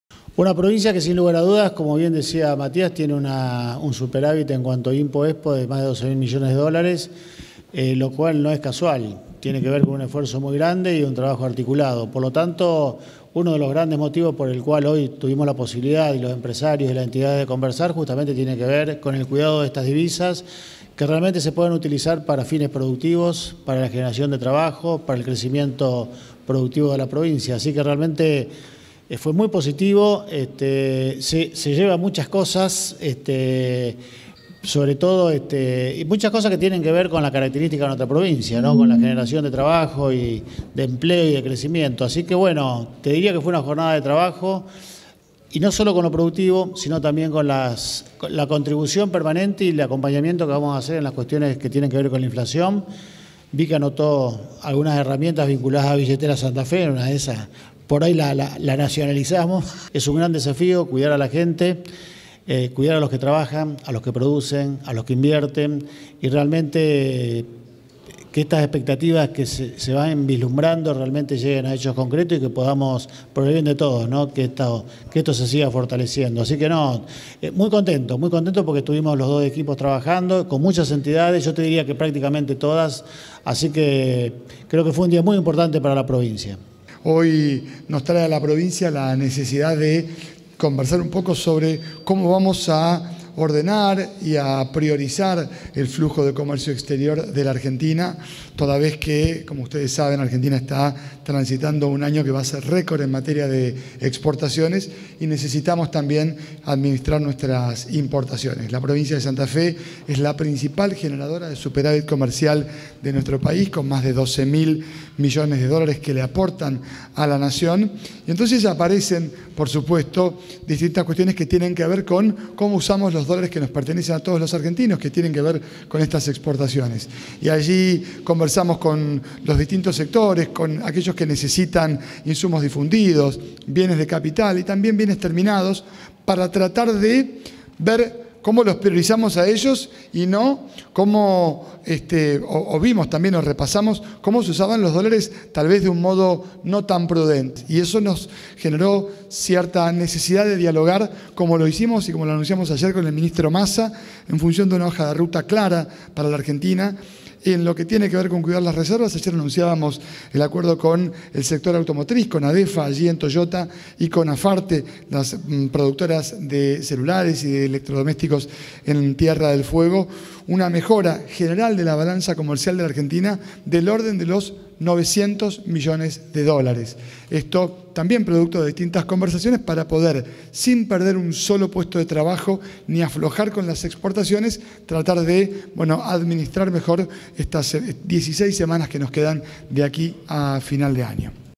Provincia y Nación abordaron aspectos comerciales junto a entidades del sector
Con la presencia del secretario de Comercio de la Nación, Matías Tombolini, se impulsó un encuentro del que participaron representantes de 26 entidades ligadas al comercio y la industria.